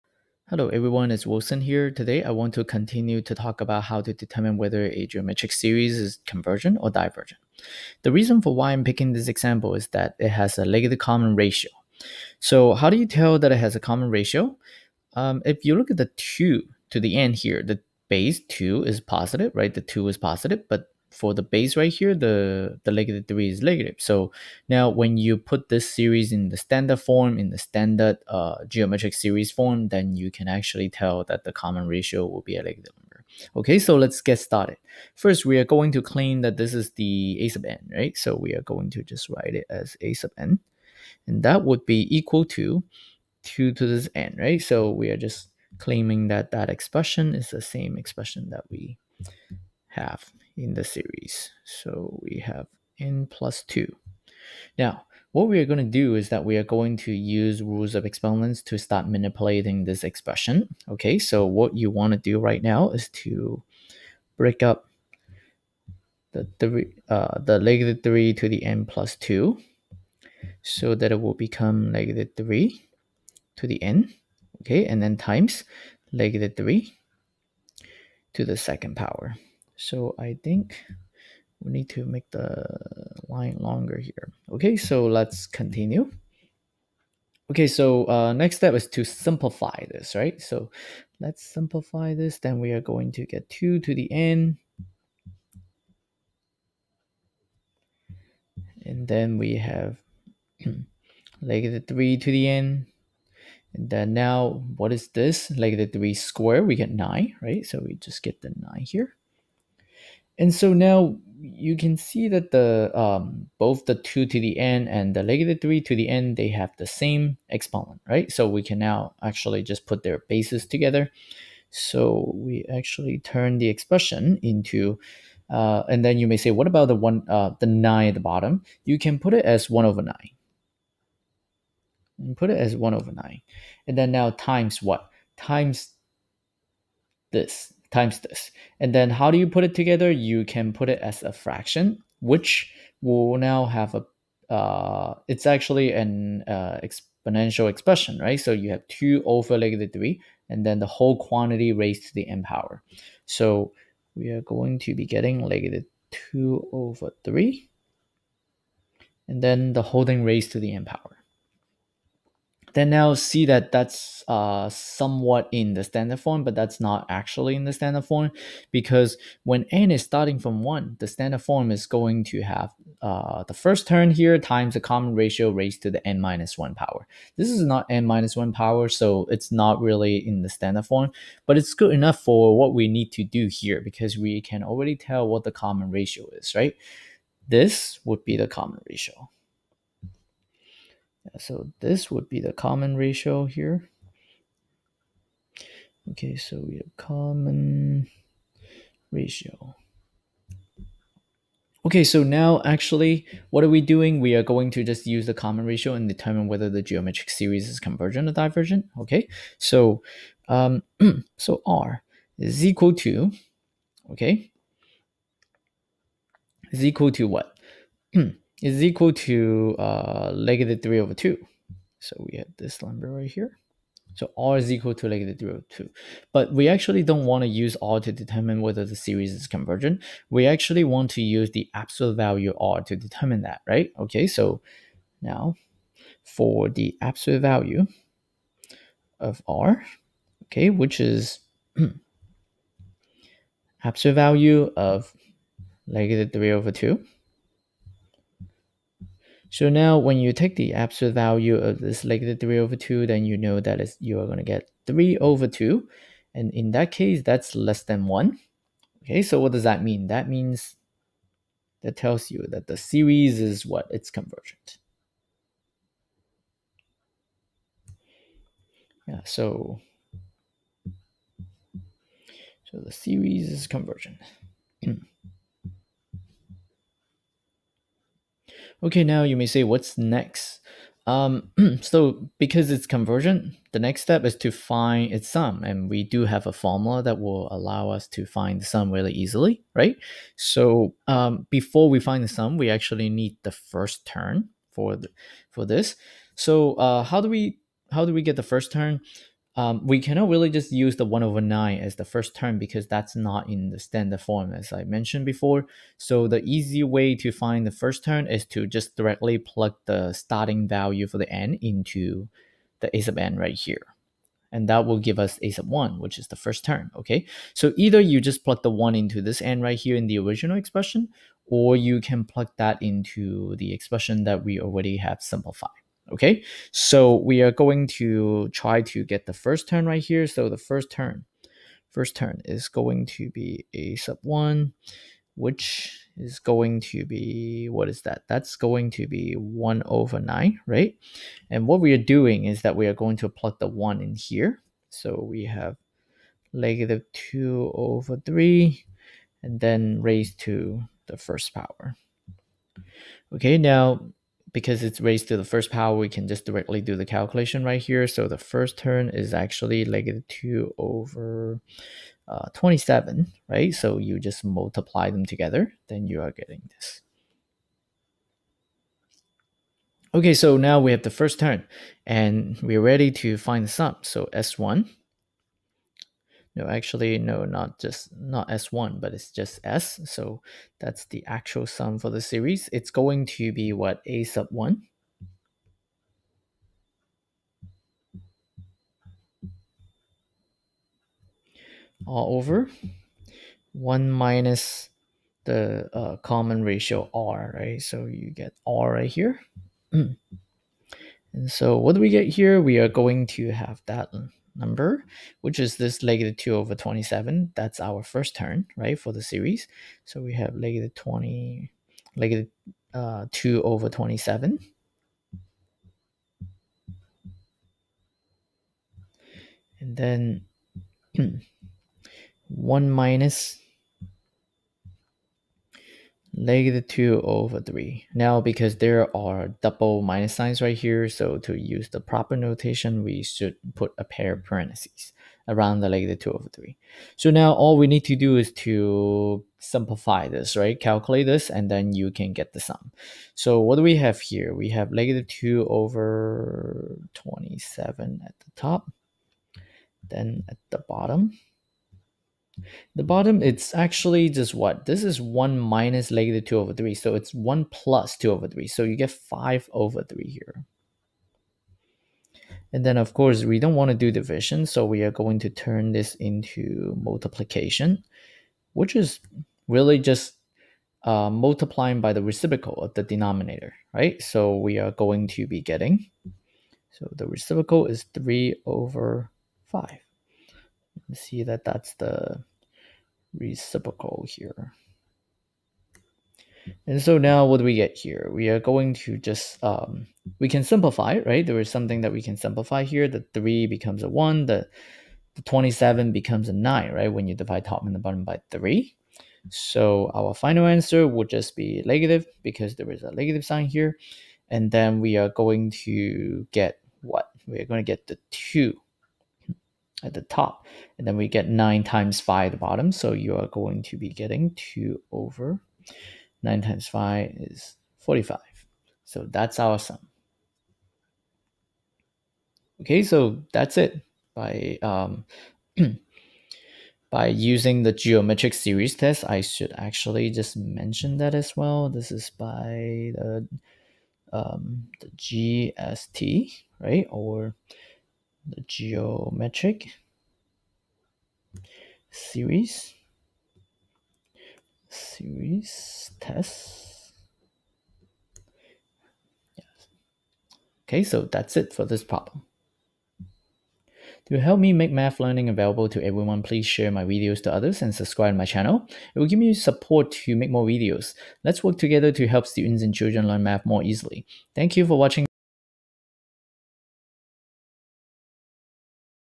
Geometric Series Test with Negative Common Ratio and Finding the Sum | Glass of Numbers
In this video, we are using the Geometric Series Test to show that a series with a negative common ratio is convergent. We also find the sum by using the formula for a convergent geometric series.

I want to use my free time to make a lot of math videos to help others and to make education available to the world.